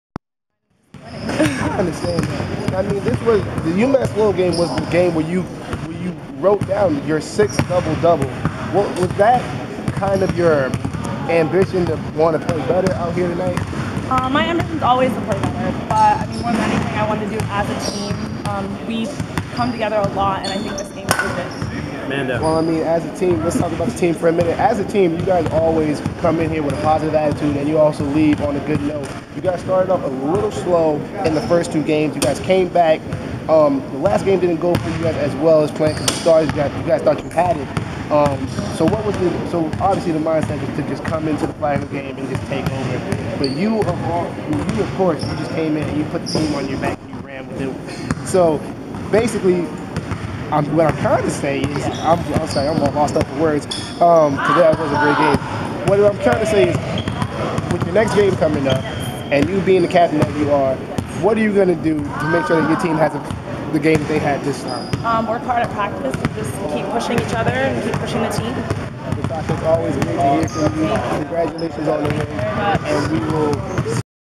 Natasha Ray on 14-Kill Outing Against Lowell
Natasha Ray led the way for Ravens volleyball with 14 kills on Tuesday in a 3-2 win over Massachusetts Lowell in the team's Northeast-10 opener. After the game, she talked to Ravens Sports Network about her performance.